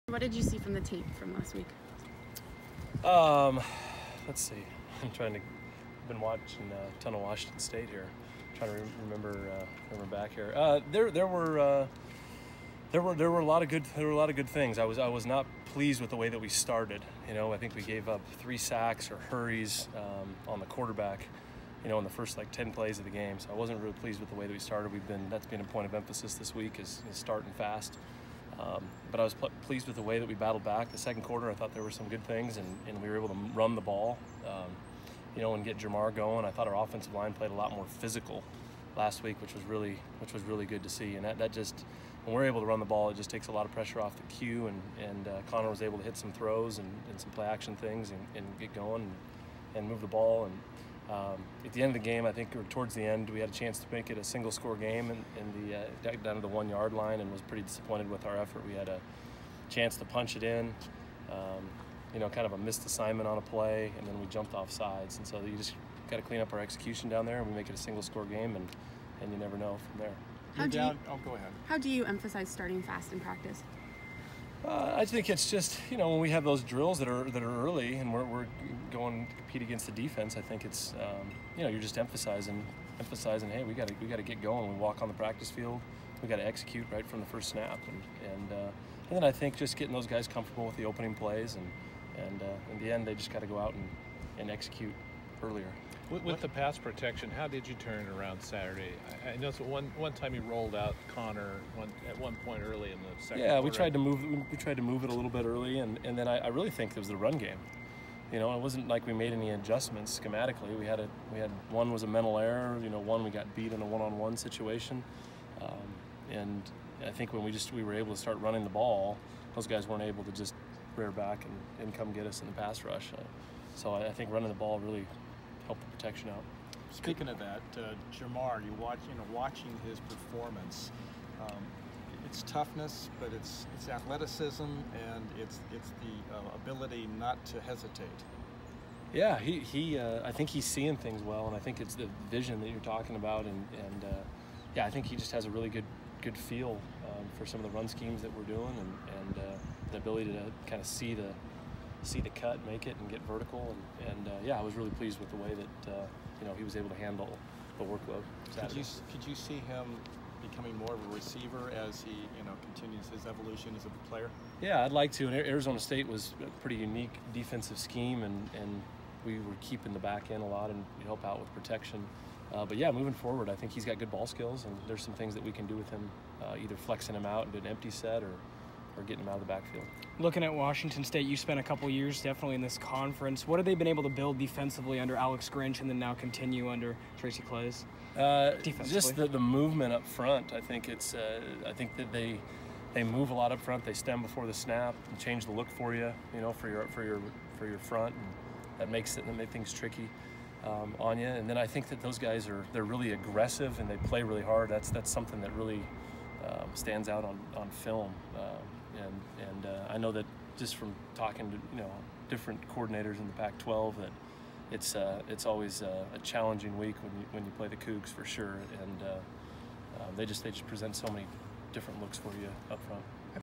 Oregon State OC Brian Lindgren on WSU
Oregon State Beavers offensive coordinator Brian Lindgren met with the media on Tuesday as the Beavers prepare to take on Washington State.